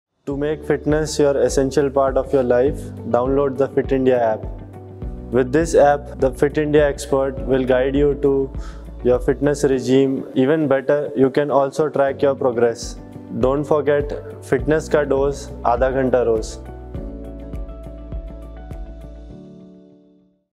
CWG Medalist Lakshya Sen shares his fitness secret
Arjuna Awardee & Badminton player Lakshya Sen makes a pledge to download the Fit India app to keep your fitness in check.